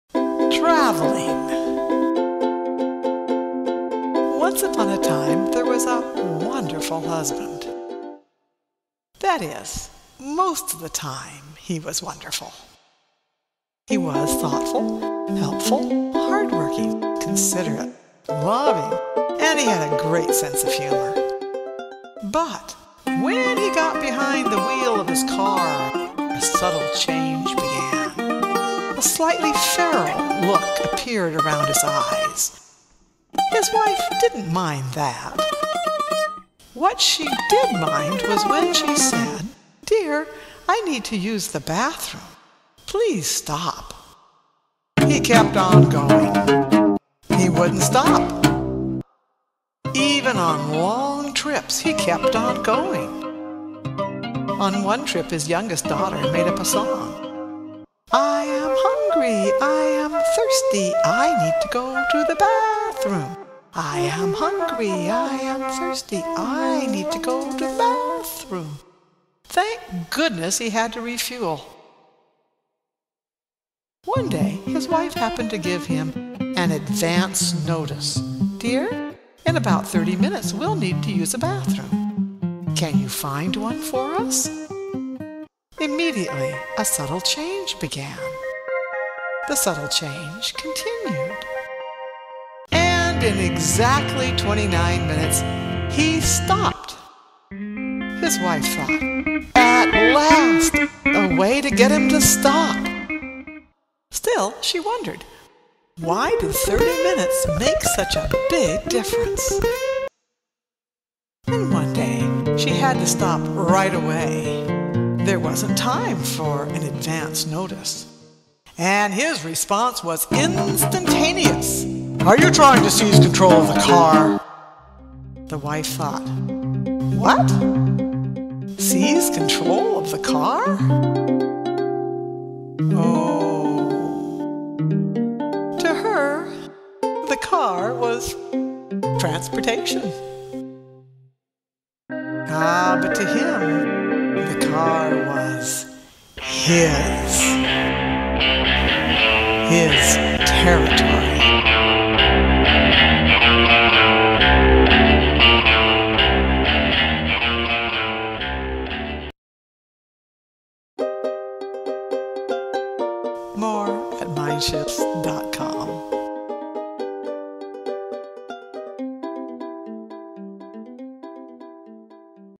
Traveling: Riding with my husband proved a lesson in communications and definitions.
After so many years married -- I still am amazed at how different the genders are.